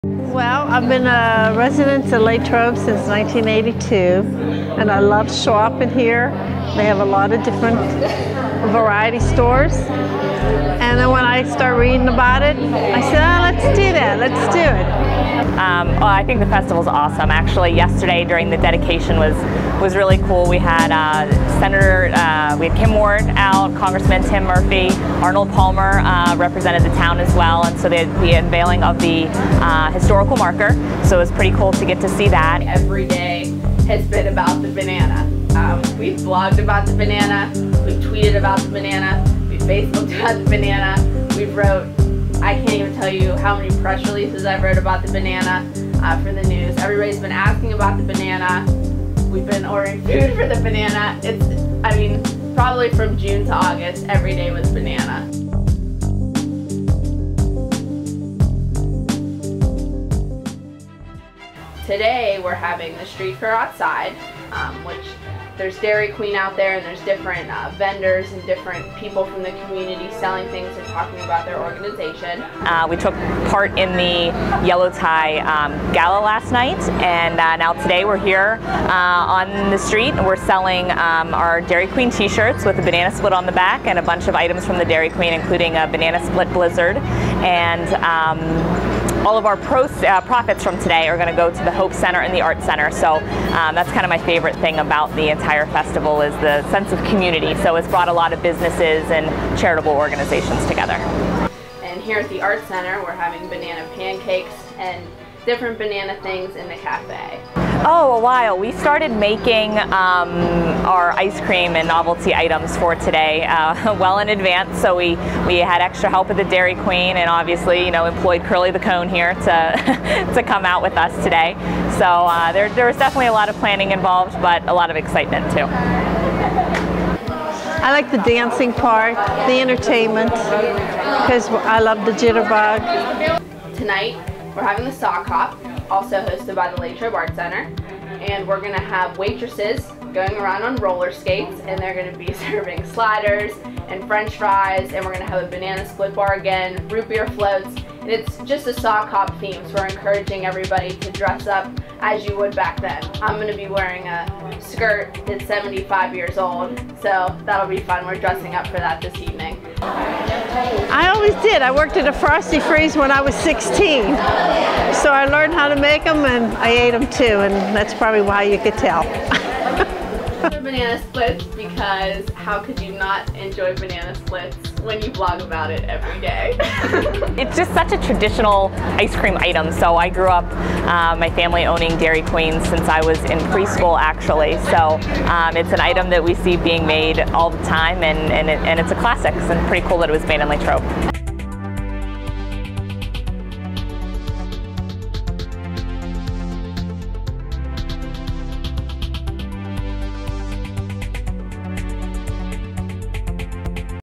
Latrobe Banana Split Festival
The Duquesne Duke spent the day at the Latrobe Banana Split Festival celebrating the home of the first banana split.